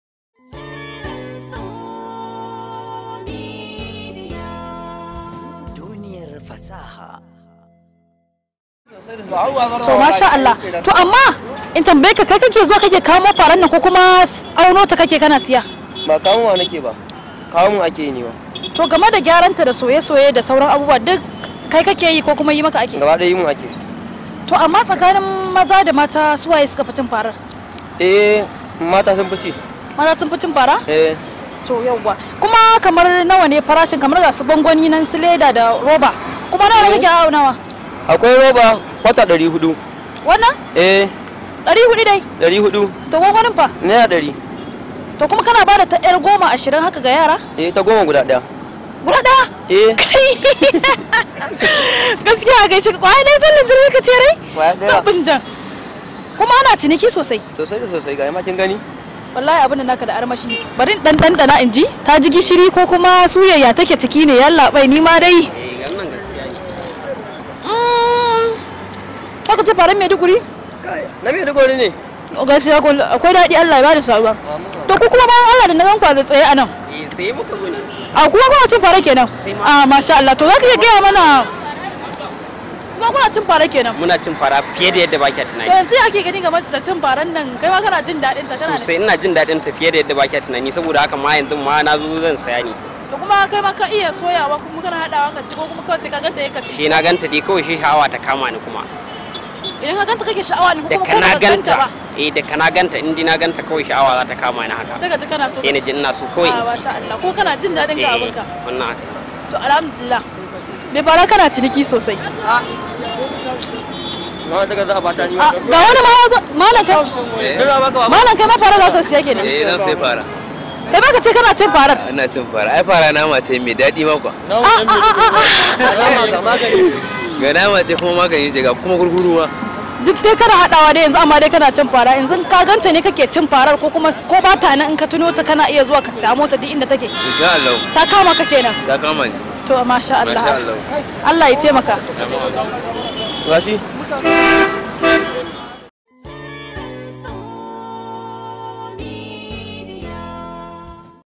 YUNWA KO MARMARI: CINIKIN SOYAYYIYAR FARA A KANO
WHY PEOPLE EATS GRASSHOPPERS?